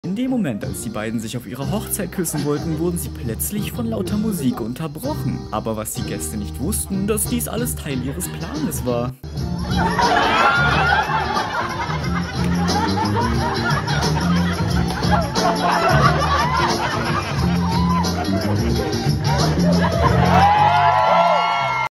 Sie Pranken ihre Hochzeitsgäste
TT Video 👉@lolhannahwhy

Indem Moment als die beiden sich auf ihrer Hochzeit küssen wollten,

wurden sie plötzlich von lauter Musik unterbrochen

aber was die Gäste nicht wussten,

dass dies alles Teil ihres Planes war :D